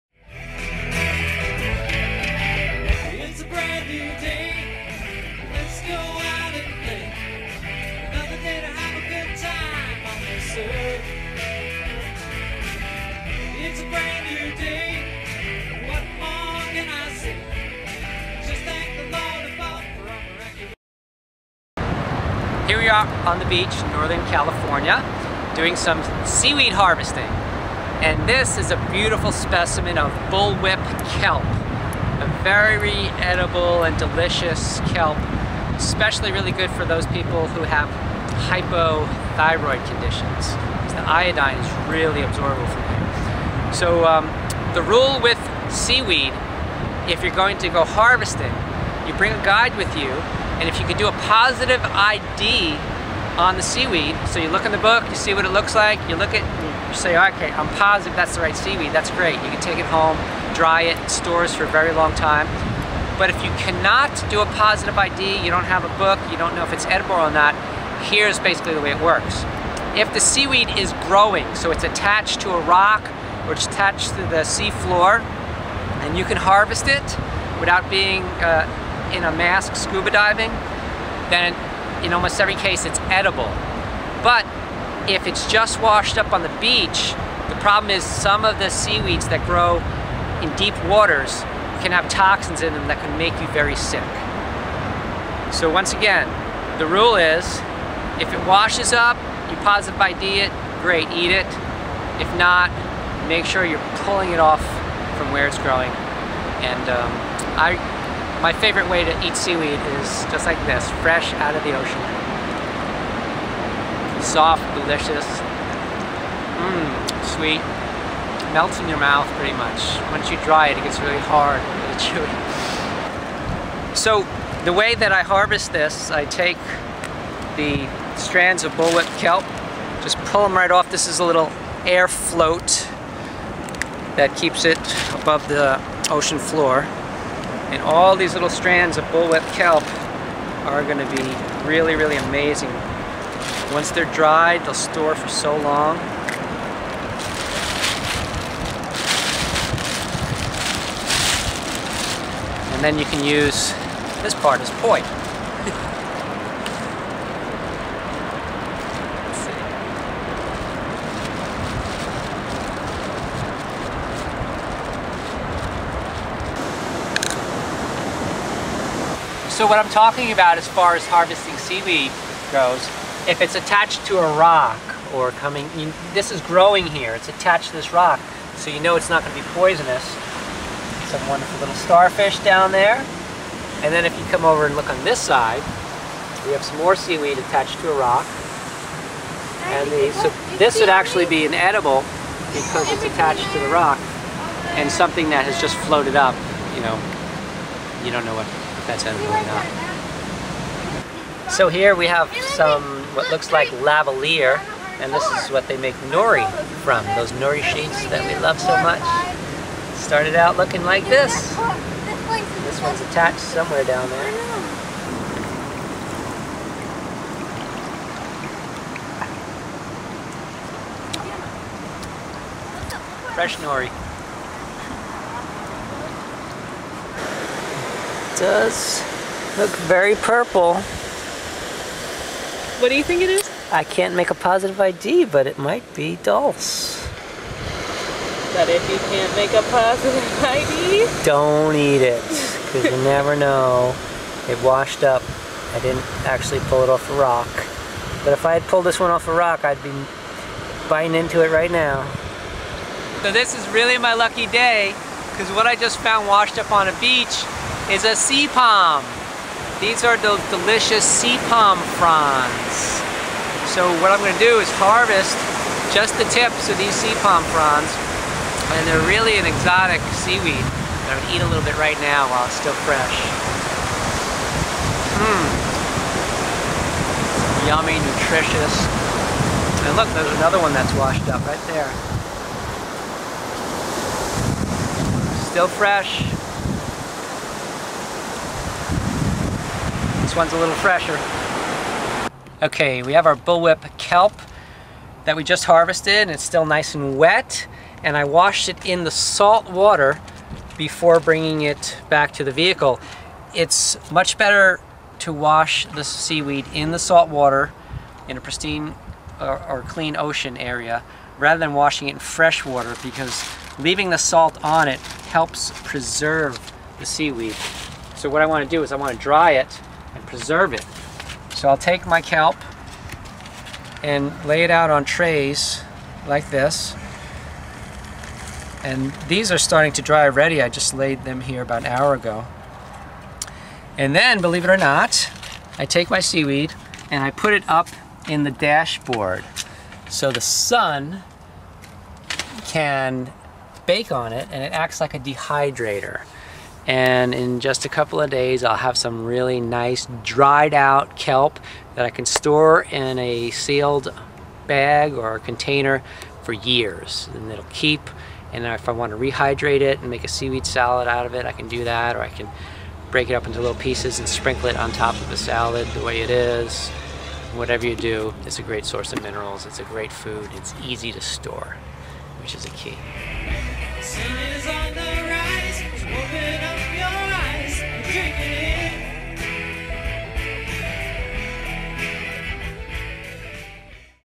Seaweed Harvesting
Take a walk with Dr Sommers on the beach and learn about harvesting sea vegetables and how to store them! Also lean how to tell edible from non edible seaweed!